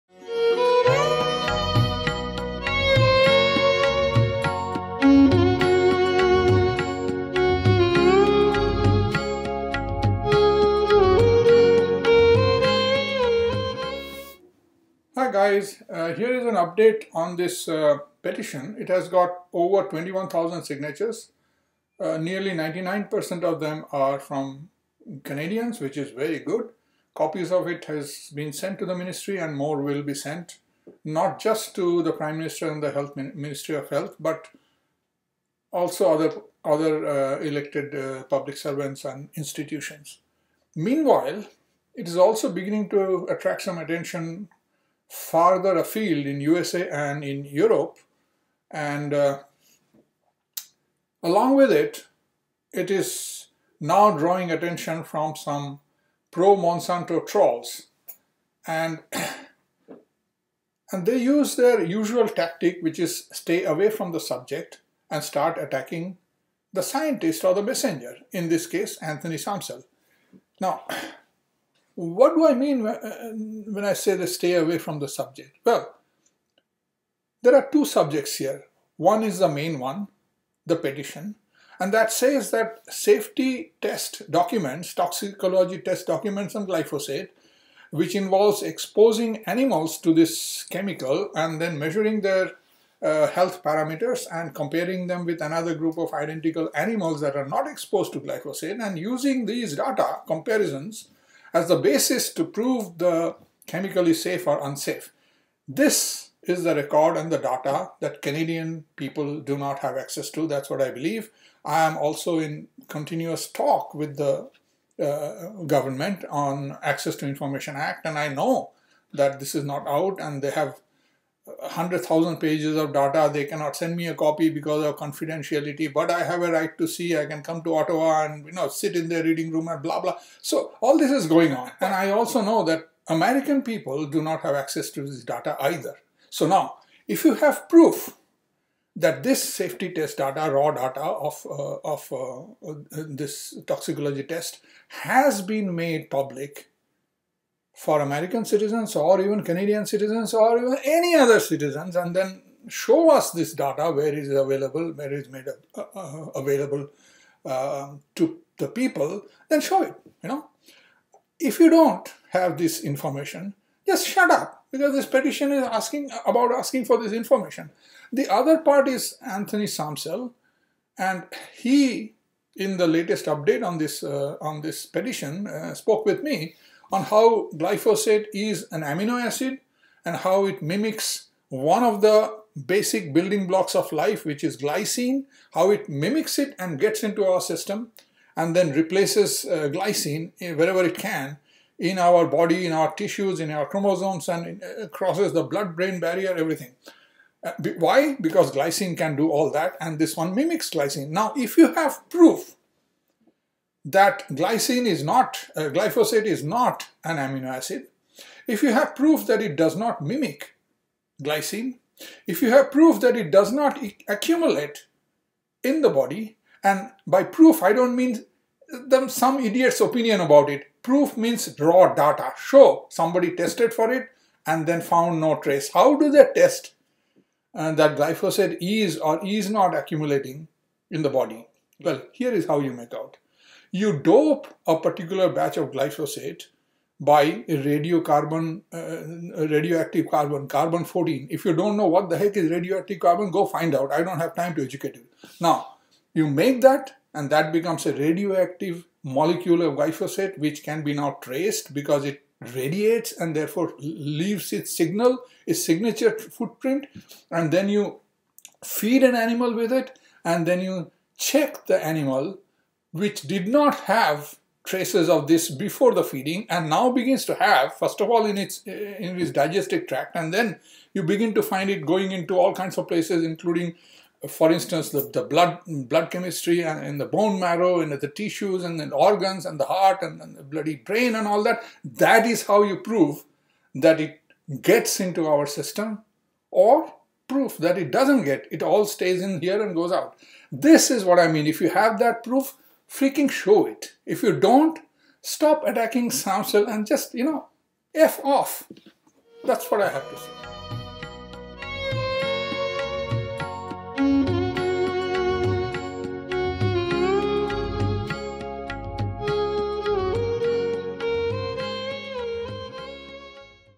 Glyphosate safety test data - petition update
The video describes it all.
There is a petition, demanding that the Government disclose hidden data on safety test conducted on animals exposed to glyphosate. People believe that the chemical is not at all safe and that is why the safety test is kept out of reach of the public.

The 21,000 plus supporters have drawn attention of a few Pro-Monsanto trolls and jackasses that veer from the subject and start trashing the messenger or the scientist.

This video is telling them to either bring proof or to get lost.